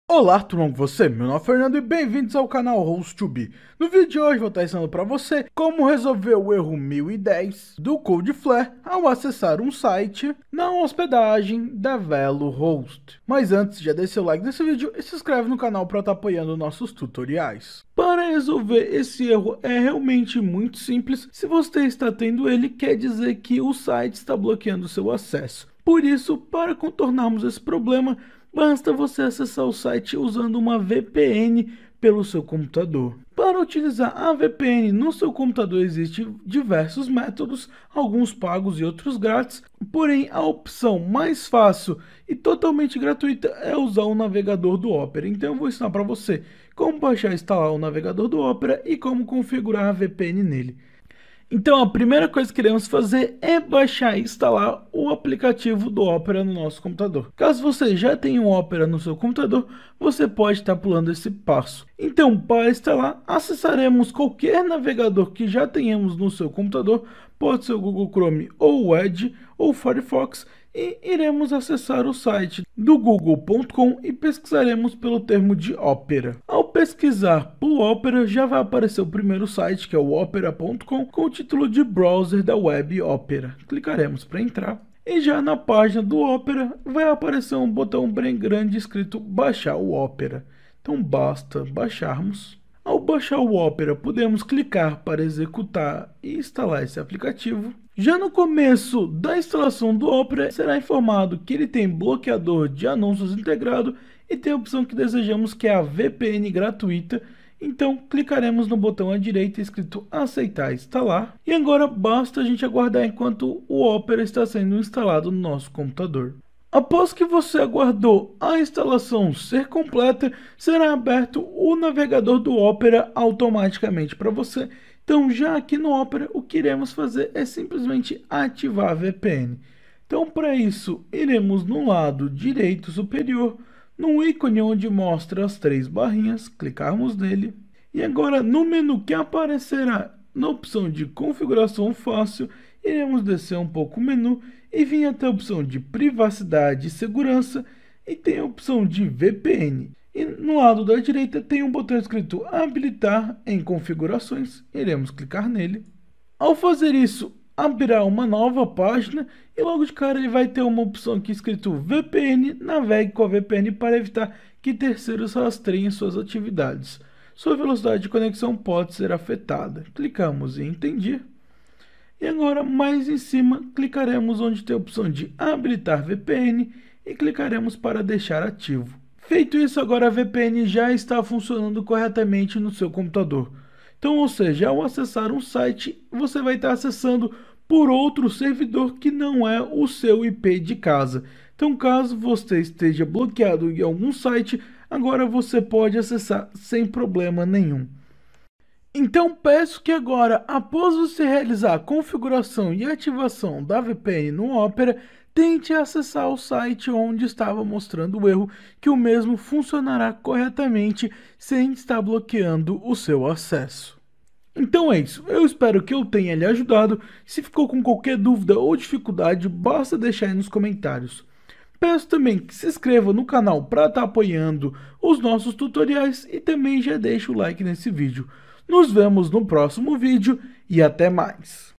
COMO RESOLVER Erro 1010 do Cloudflare na Valuehost – rápido e fácil – 2022
Aprenda Como Resolver o erro 1010 do Cloudflare na Valuehost facilmente e em poucos minutos

LINKS IMPORTANTES!!!